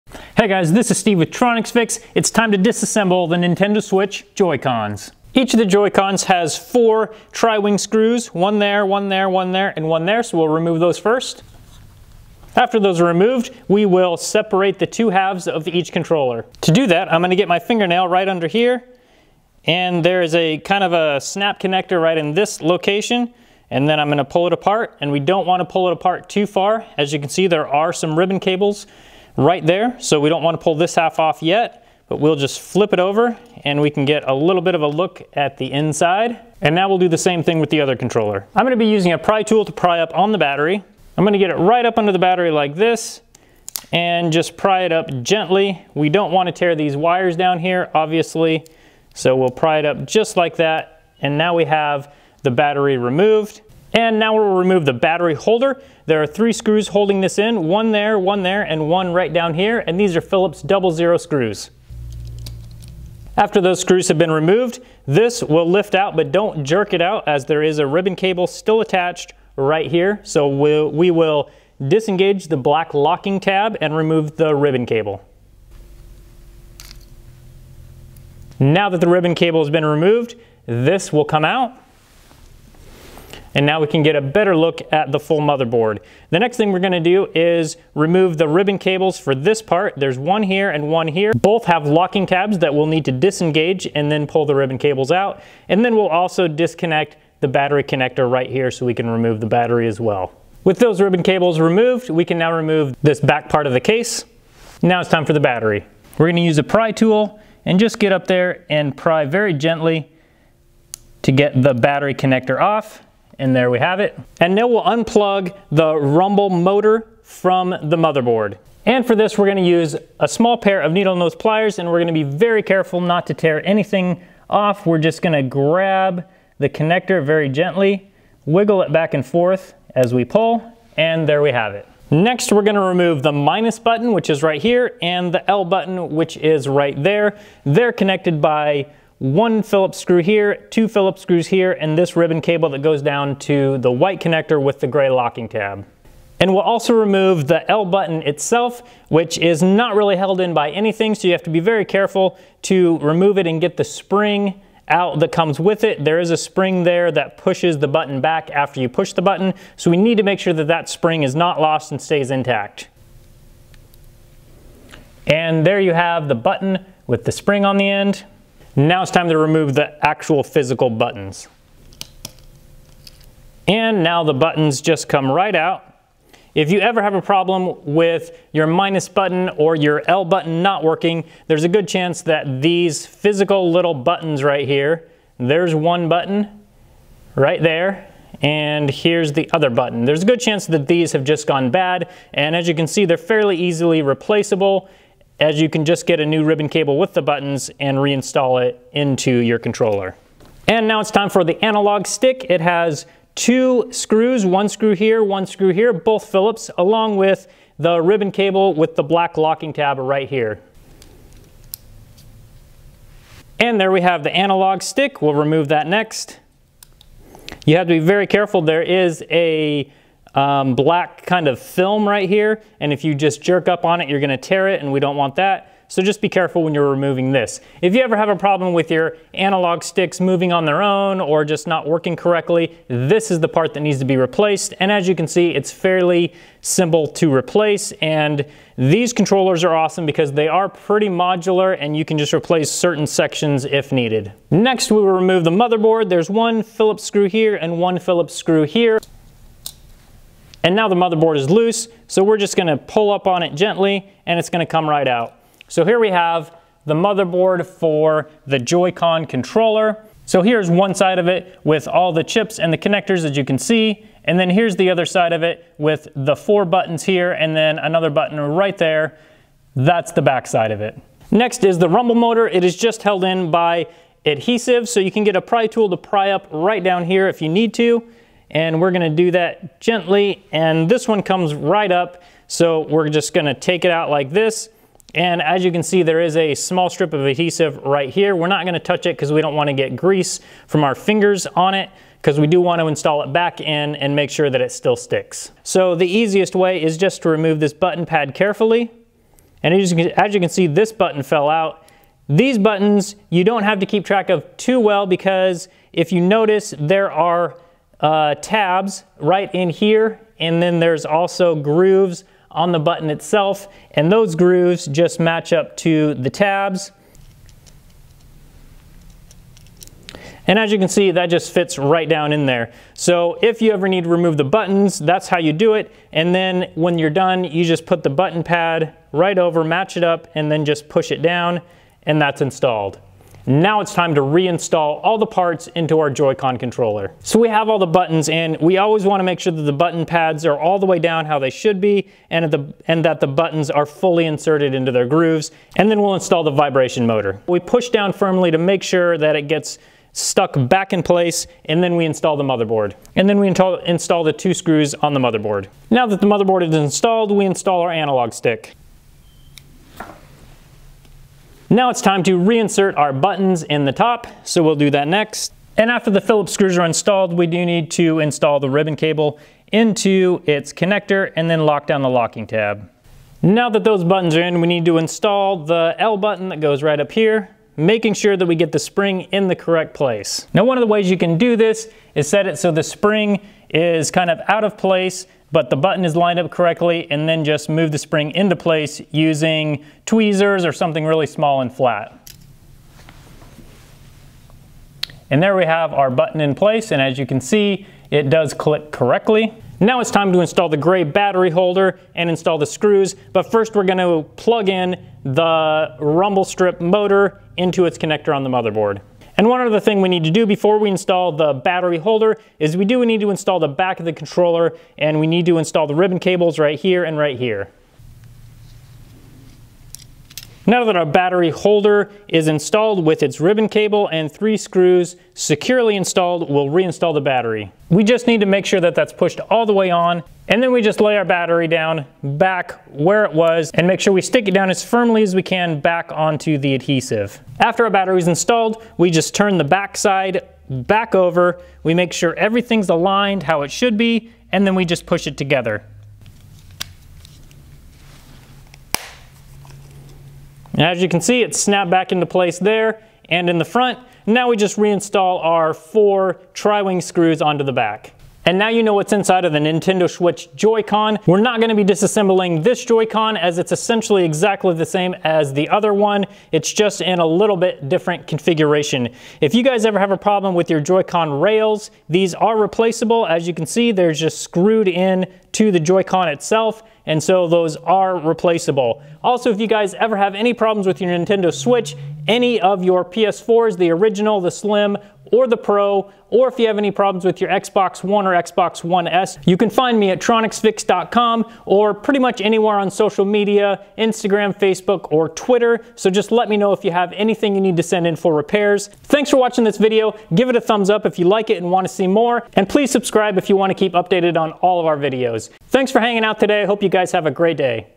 Nintendo Switch Joycon Teardown -
I disassemble the Nintendo Switch Joycon controller to see what makes it tick. After I completely disassemble it I show how to reassemble it without breaking anything.
First the screws on the back need to be removed, then the shell just has to be snapped out of place. There are several ribbon cables still attached that are easy to break. After those ribbon cables have been removed I remove the battery and battery holder. Then the analog stick. After that the motherboard comes out. I then remove the new style vibration motor which is held in place by an adhesive strip.
While torn down I show the internal parts of the Joycon controllers and find that they are very modular and should be fairly easy to replace parts if/when needed. These Nintendo Switch controllers look to be built well and should last quite a while.